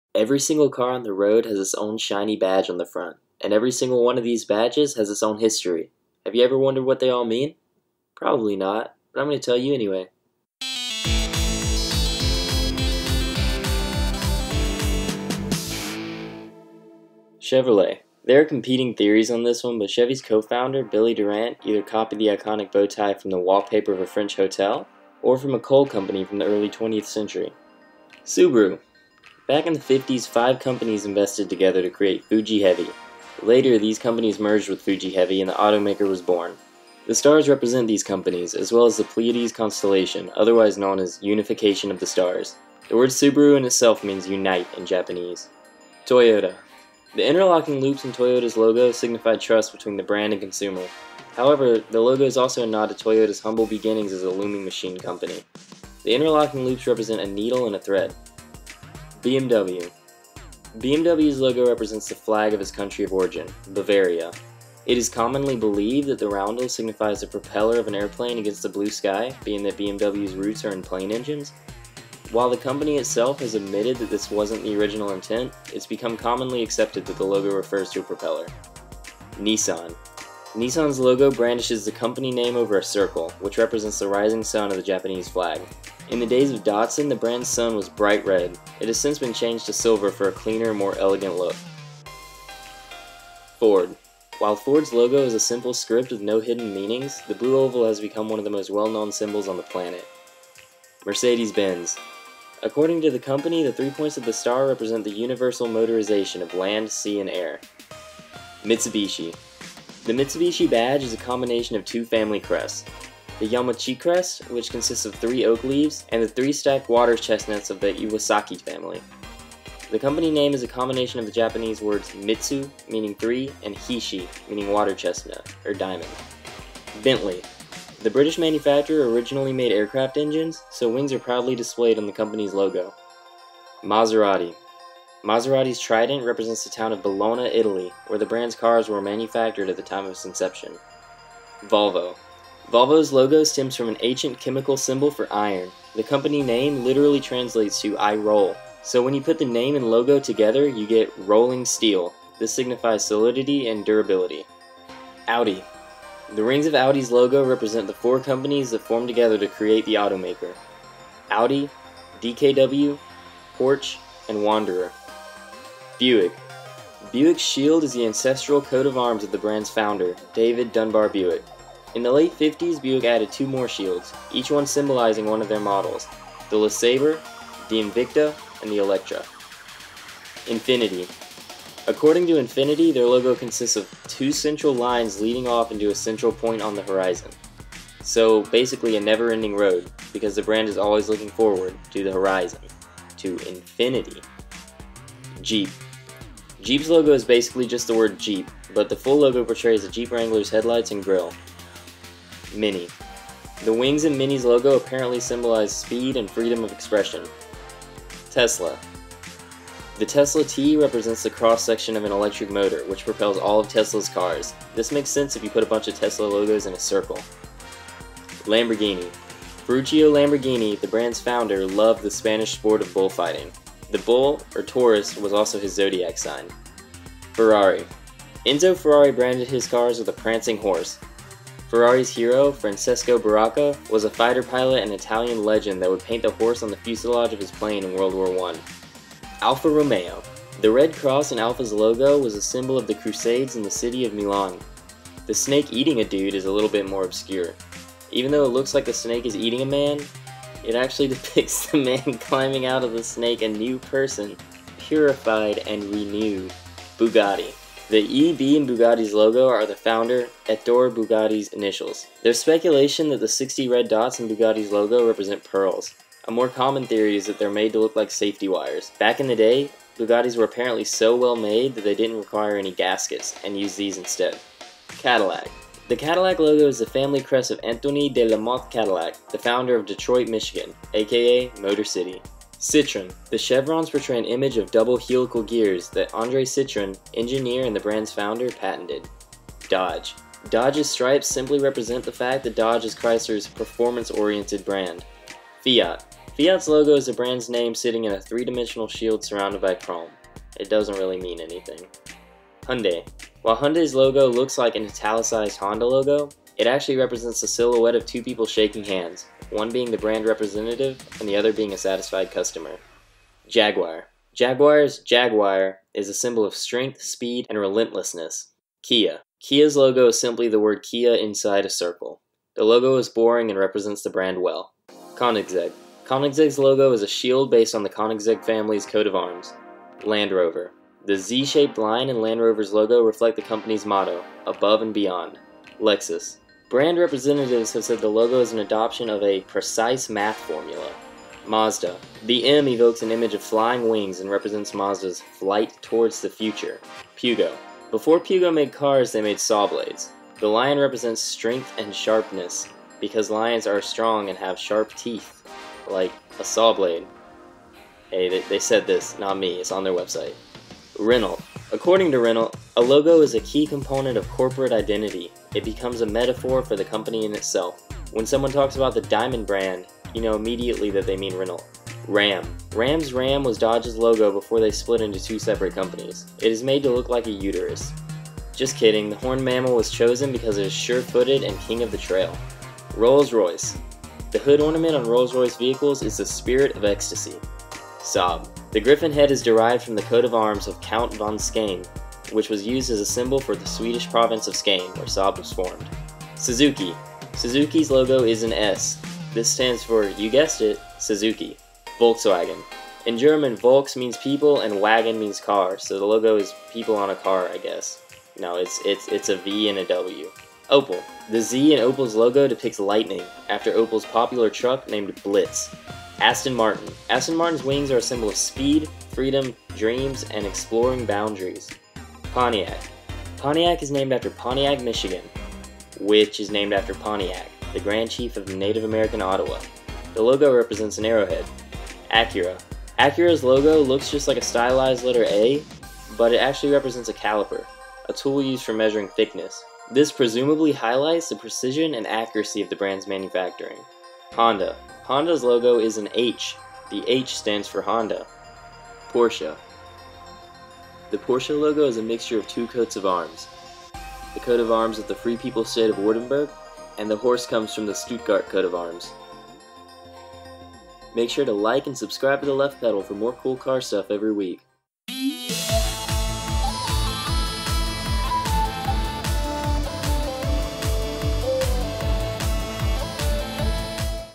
The Secret Meaning Behind EVERY Car Logo
Every car has a logo, and every one of those logos means something. Buckle up, we're going to discover the HIDDEN meanings behind practically every single automobile brand out there.

We made sure to get the right information through heavy research and contacting companies to discover the truth behind these secret logos. Brands intentionally place subliminal messages in their products so that consumers can easily recognize them.